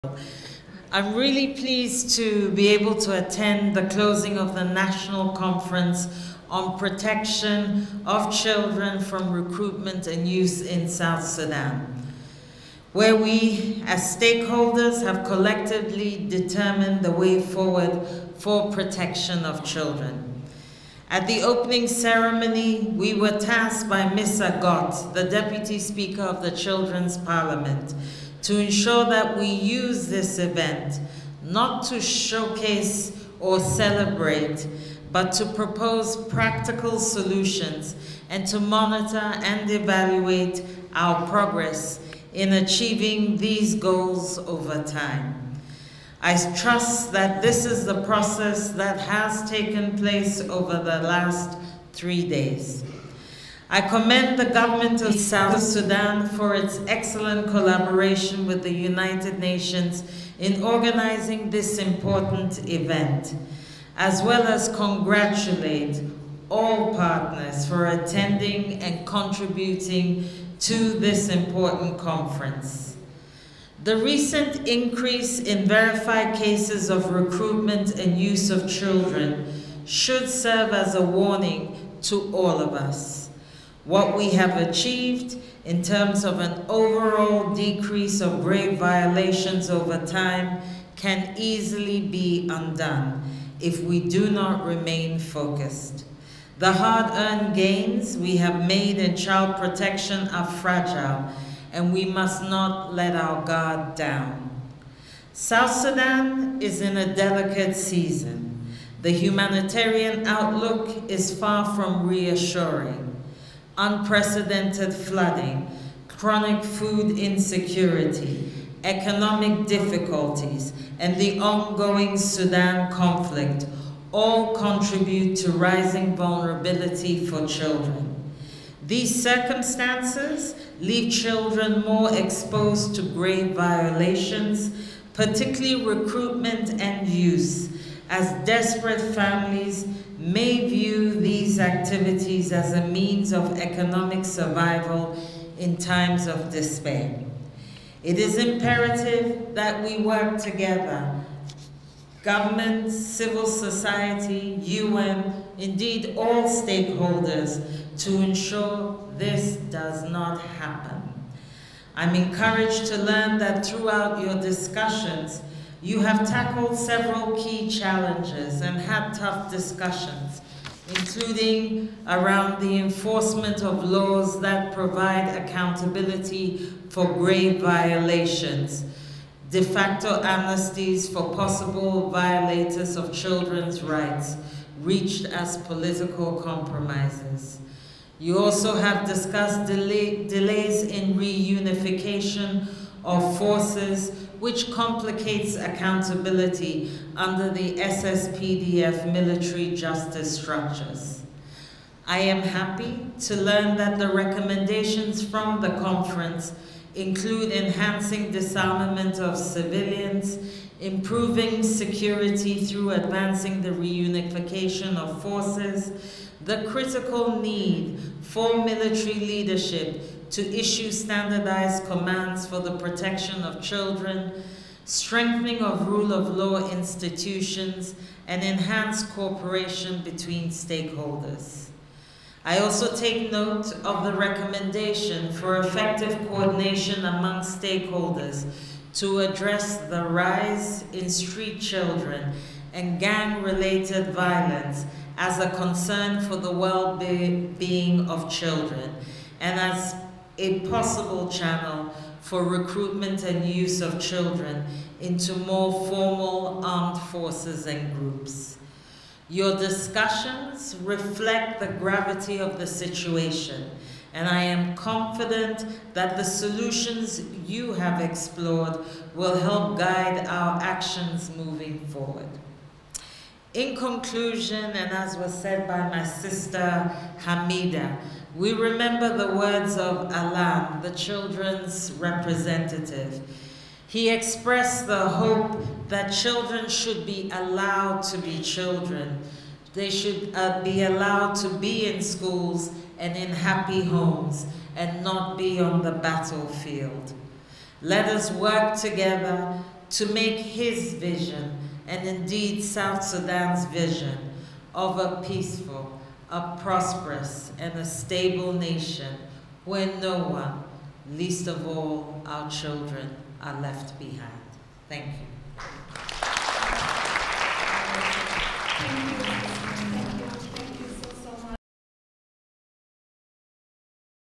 3rd National Conference on the Protection of Children against Recruitment closes after three days
Kiki Gbeho - the Deputy Special Representative of the Secretary-General and Resident and Humanitarian Coordinator for the United Nations and co-chair of the Country Task Force of the Monitoring and Reporting Mechanism (CTFMR) in South Sudan, takes the stage and highlights gains made at the 3rd National Conference on the Protection of Children against Recruitment and Use, between 17 to 19 September 2024, organized by the Government of South Sudan in collaboration with the United Nations.